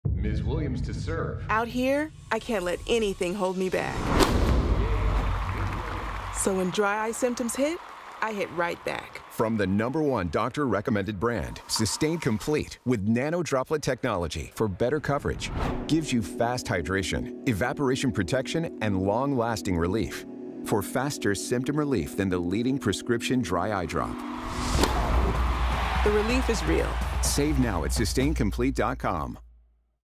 SYSTANE® COMPLETE Lubricant Eye Drops: Get Real Relief
There’s never a good time to experience dry eye symptoms, especially when they prevent you from doing what you love most. Hear why professional tennis player, Venus Williams, turns to SYSTANE® COMPLETE so she can be at peak performance on and off the court. Learn more about SYSTANE® COMPLETE eye drops